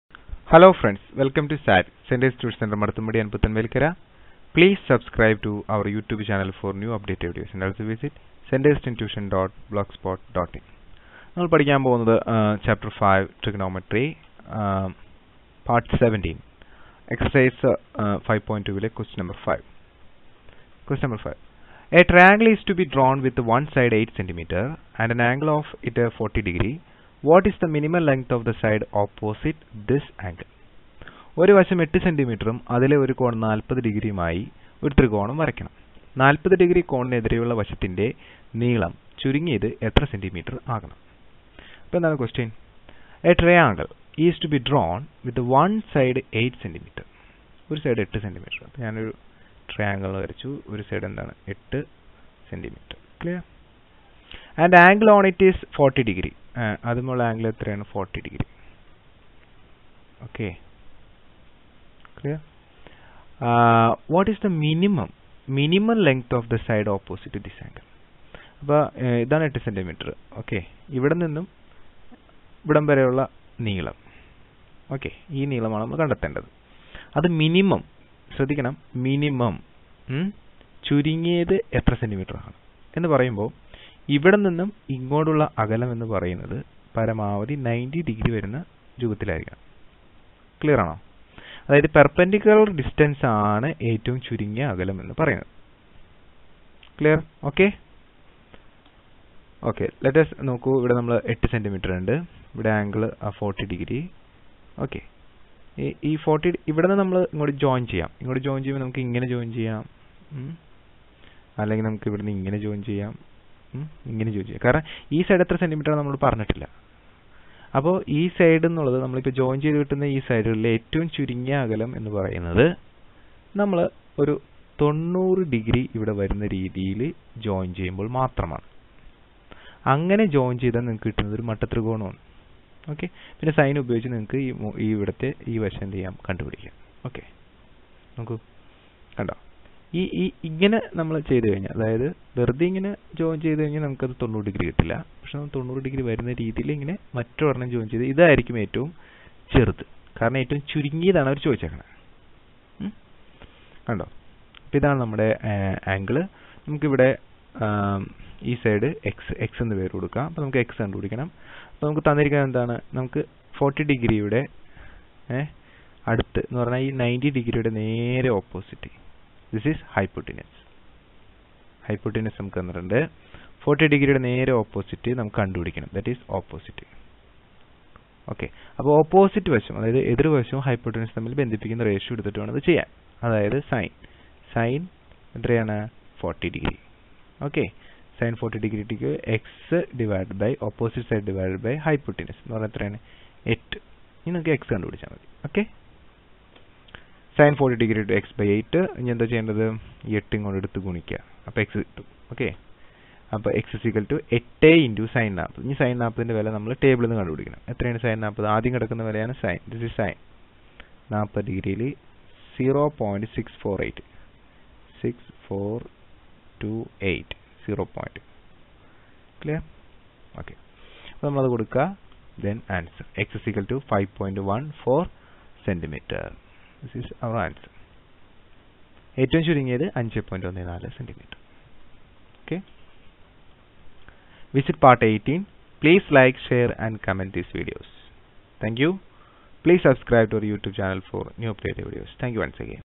MATHS| PART - 17 Ch.5 -Trigonometry |Text Ex.5.2 Q5| 2016| CLASS 10 KERALA
Purchase E book GO
Learn SSLC MATHS class 10 KERALA (NEW SYLLABUS)
chapter 5 TRIGONOMETRY -. This video contain Textbook Exercise 2 Question 5- in trigonometry- chapter 5 of maths kerala sslc 2017.
hosted by SUNNY THOMAS CT